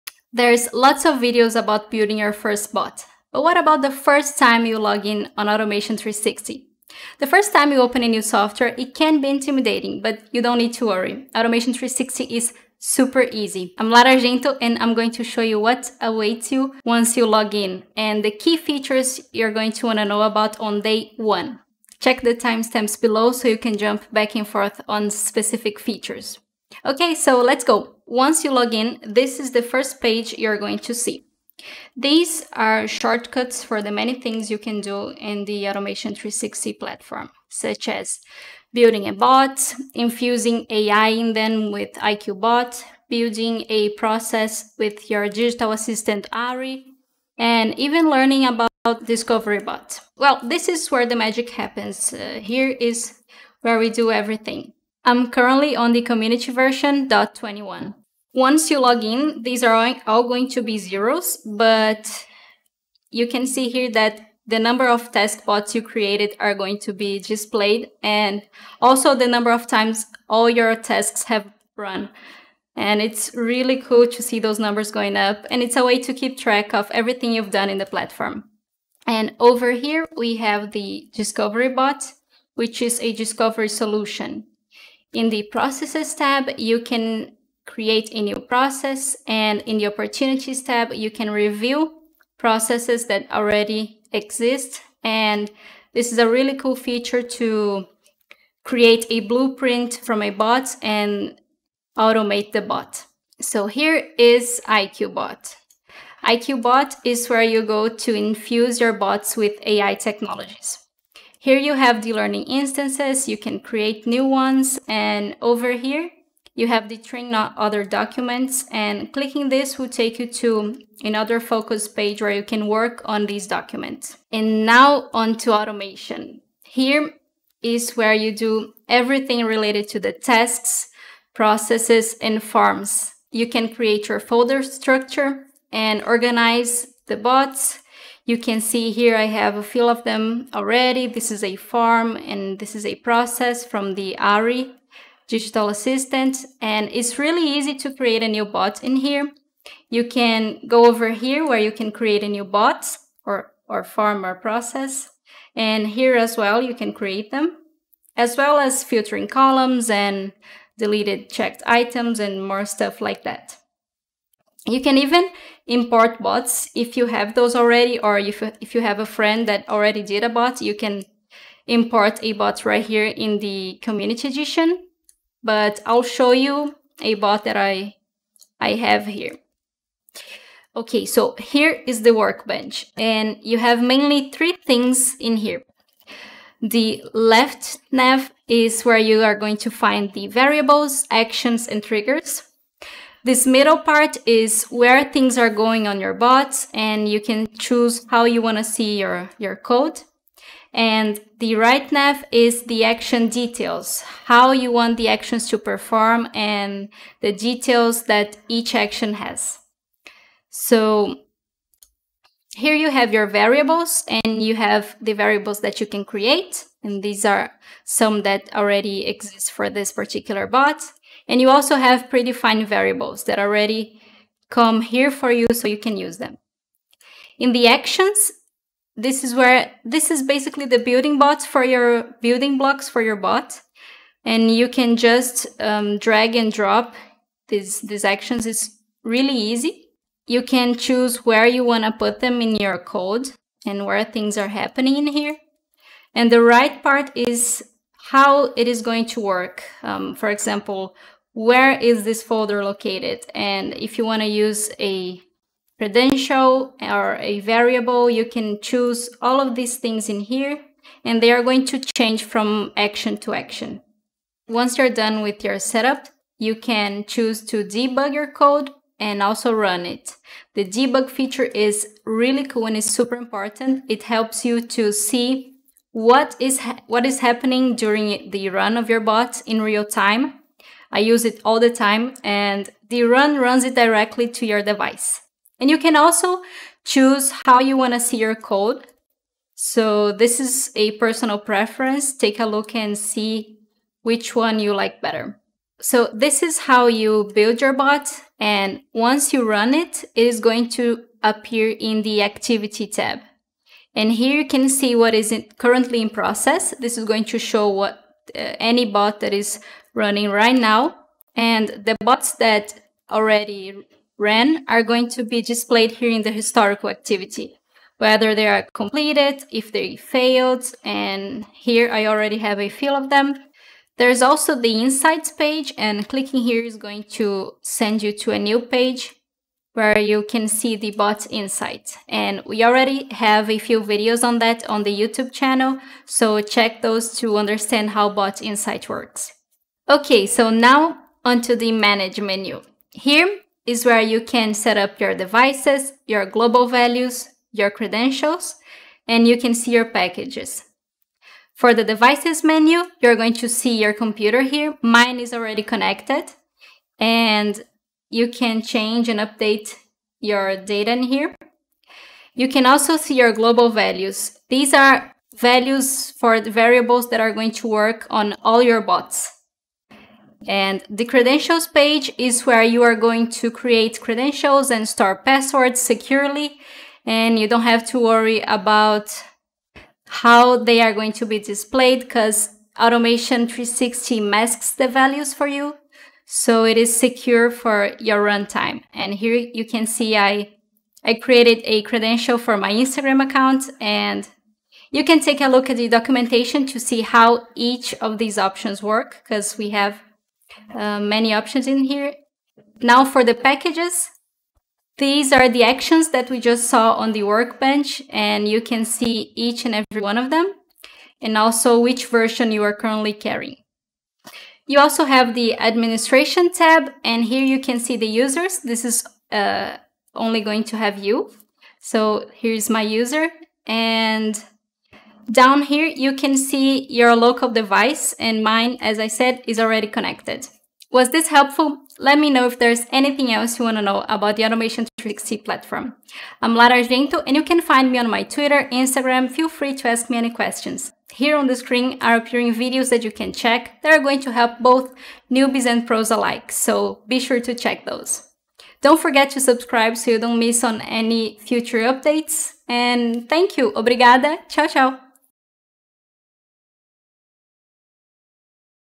How to Get Started with the Automation 360 UI with Lara Argento | Automation Anywhere
There are lots of videos on our channel about building your first bot, but what about videos about opening Automation 360 for the first time? Join Lara Argento, Automation Engineer at Automation Anywhere, as she takes us on a quick tour of the UI and key features of the new v.21 release of Automation Anywhere 360.
Click below to jump to a particular segment:

[0:28] Explore Page 🔍
[0:57] Home Screen 🏠
[1:18] Discovery Bot 💡
[1:43] IQ Bot 🧠
[2:09] Automation Repository 📦
[3:00] RPA Workspace 🛠
[4:58] Activity Tab 📊
[5:39] Manage Menu 🙍‍♂️

Want to learn more about the incredible features in Automation 360, and how it can change the way you work?